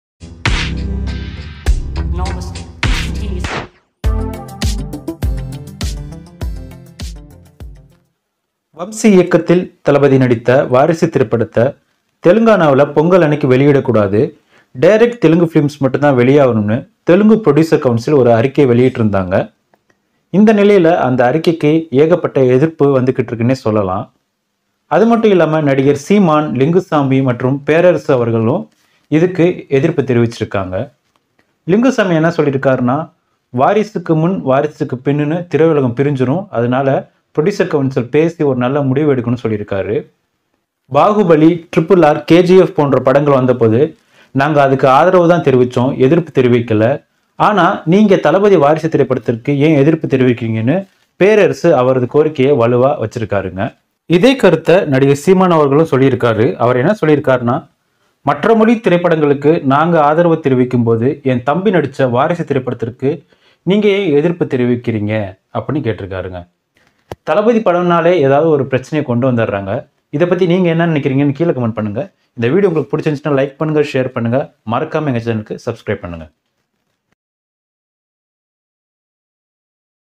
Varisu Issue | தளபதி படத்திற்கு மட்டும் பிரச்சனை எங்கிருந்துதான் வருதோ
Mr. Seeman, Mr. Lingusamy and Mr. Perarasu support for Varisu release in Telangana.